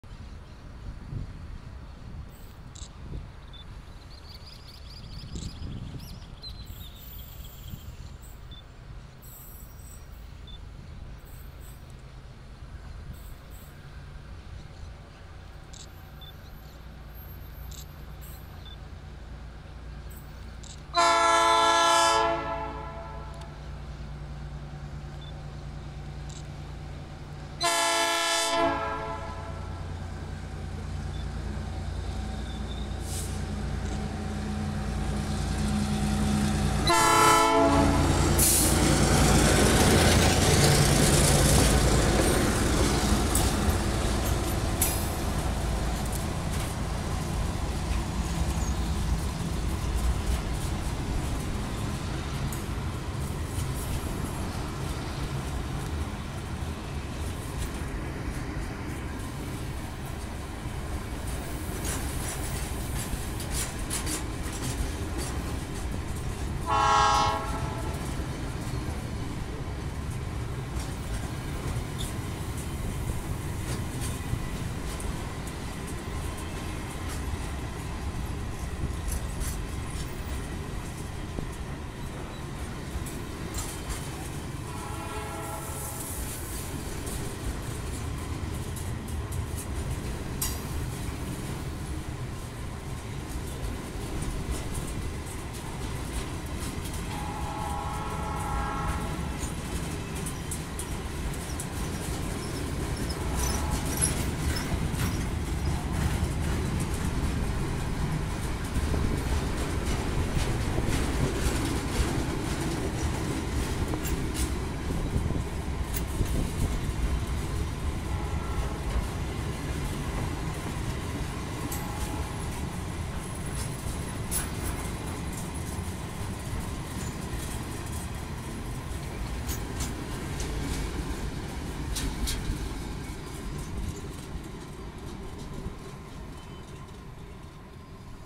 GR12 6580 haciendo unos de sus primeros viajes después de una RG. (0572) 19-7-2023
GR12 6580 haciendo unos de sus primeros viajes después de una RG. Desde principios de Noviembre del 2022 hasta el Martes pasado estuvo en reparación en los Talleres Villa Diego, el domingo ya se encontraba en Patio Parada.
GR12 6580 doing one of his first trips after a RG. From the beginning of November 2022 until last Tuesday it was under repair at Talleres Villa Diego, on Sunday it was already in Patio Parada.